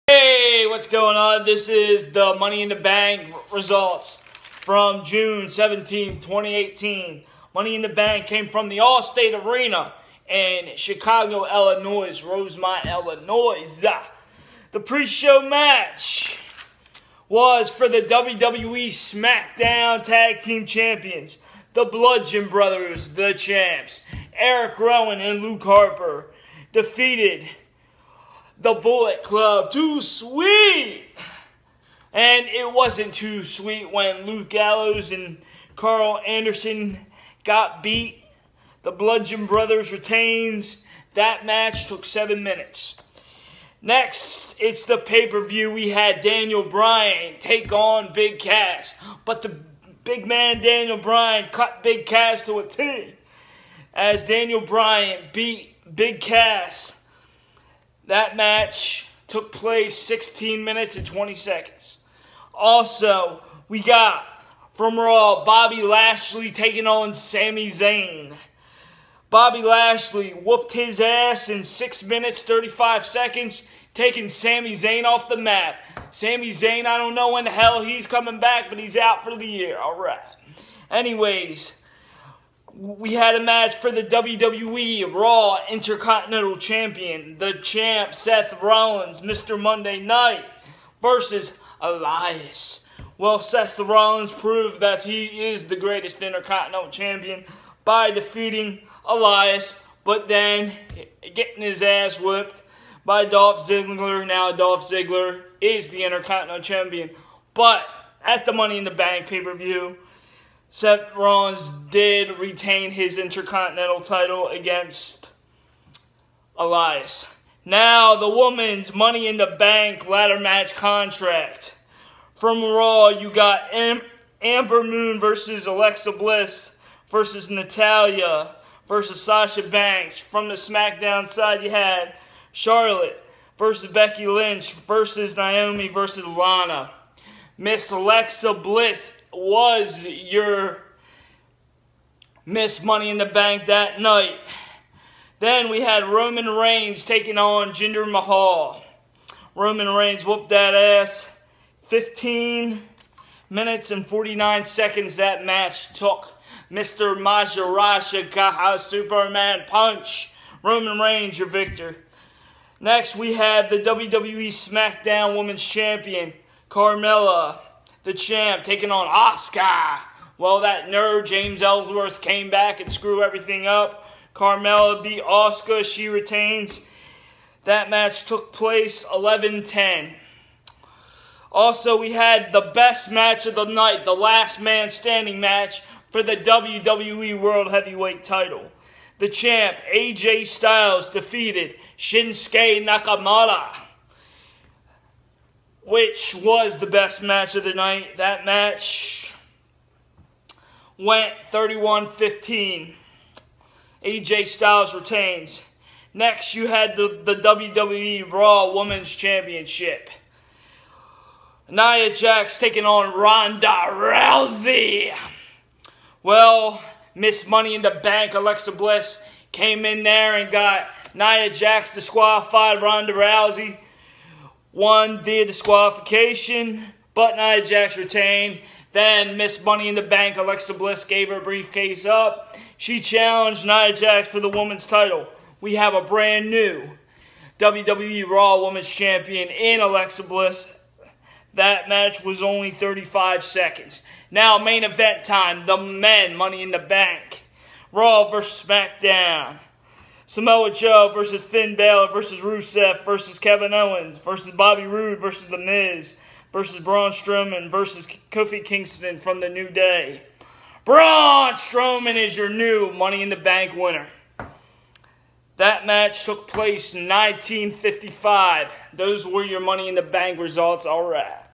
WWE money in the bank 2018 results
WWE money in the bank 2018 June 17, 2018 from the Allstate Arena in Chicago Illinois here are the results